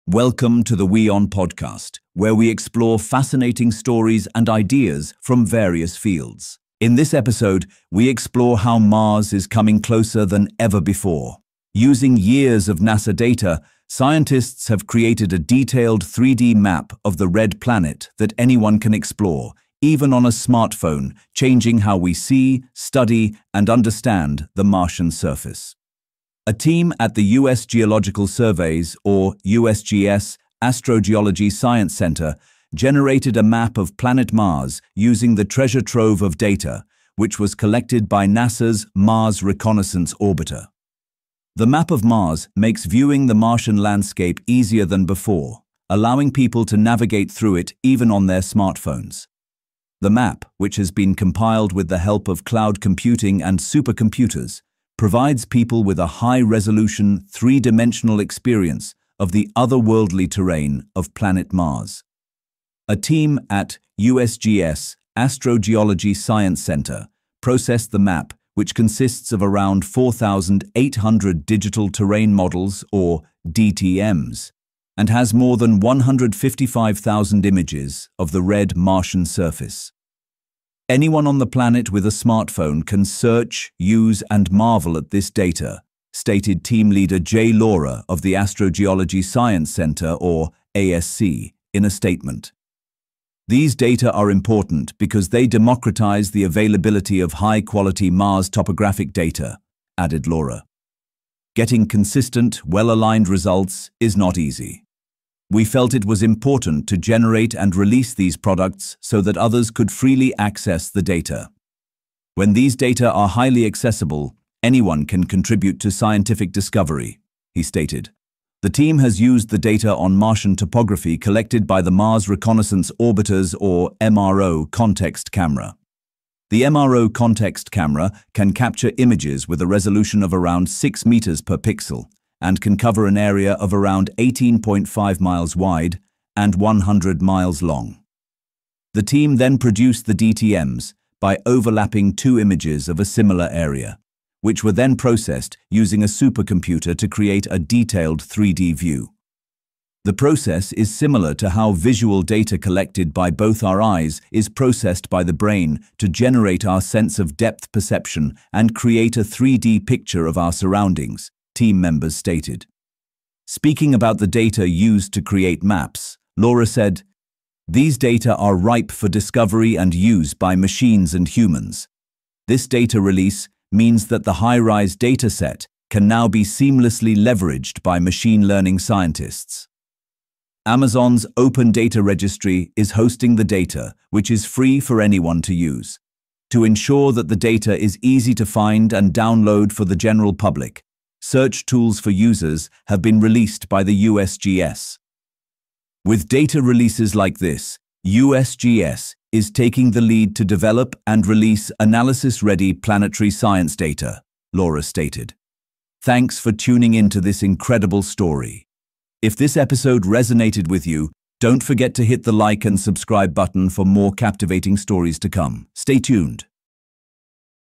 Explore Mars in 3D on Your Phone Using NASA Data | WION Podcast
Mars is closer than ever before. Using years of data collected by NASA’s Mars Reconnaissance Orbiter, scientists have created a stunning 3D map of the Red Planet that anyone can explore on a smartphone. Developed by researchers at the US Geological Survey’s Astrogeology Science Centre, the map combines thousands of terrain models and images into an immersive experience. Powered by cloud computing and supercomputers, this breakthrough is changing how Mars is studied and understood. By making high-quality planetary data freely accessible, scientists hope to unlock discoveries by both humans and machines.